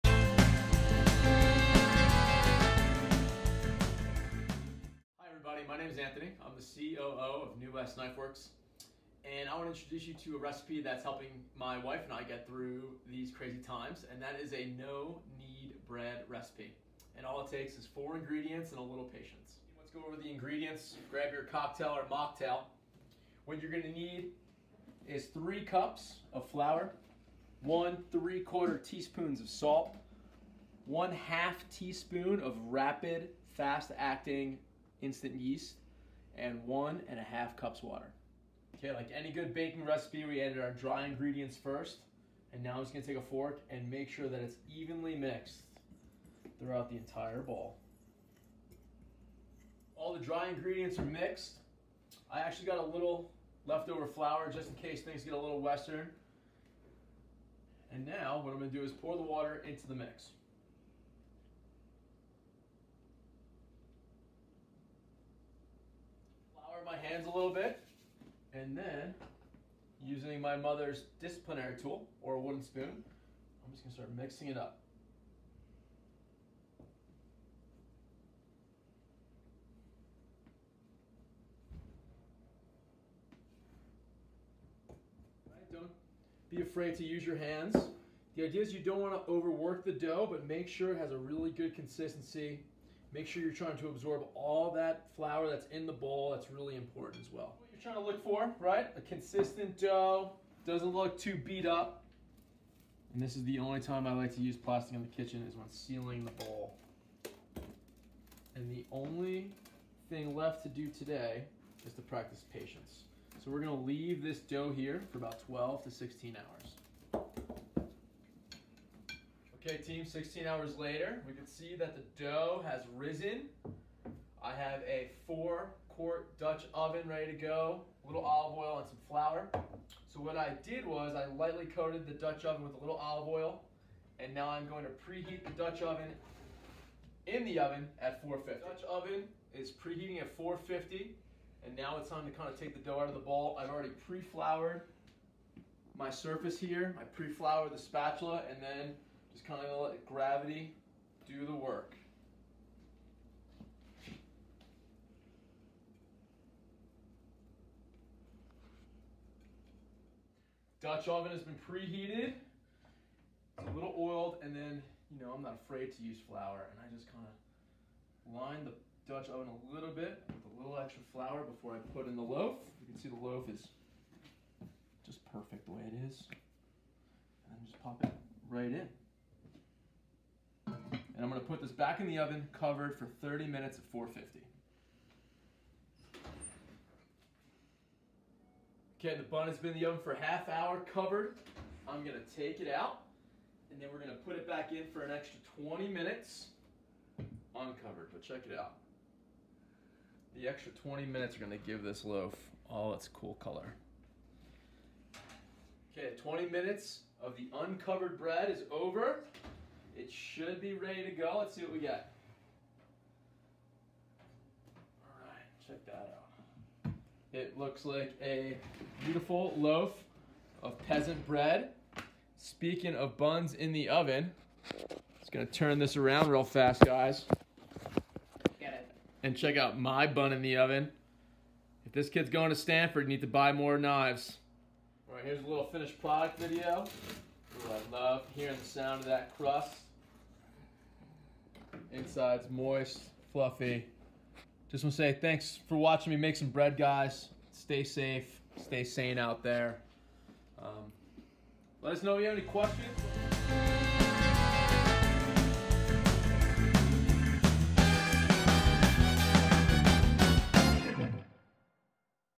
At Home with New West: No-Knead "Peasant Bread"
With more time at home, cooking has not only become a way to stay nourished, but a hobby and a pastime for more people than ever. Trying out a new recipe is a way to stay entertained and master a new skill!

The New West team has been compiling their favorites, and we're kicking it off with a surprisingly simple - but crusty and delicious - homemade bread recipe. Our COO and resident Italian-American, Anthony Campolattaro, shares an easy-to-follow recipe in this delightful video shot by his lovely (and very pregnant!) wife, Annie.

Let us know how you like the video or if you tried out the recipe in the comments!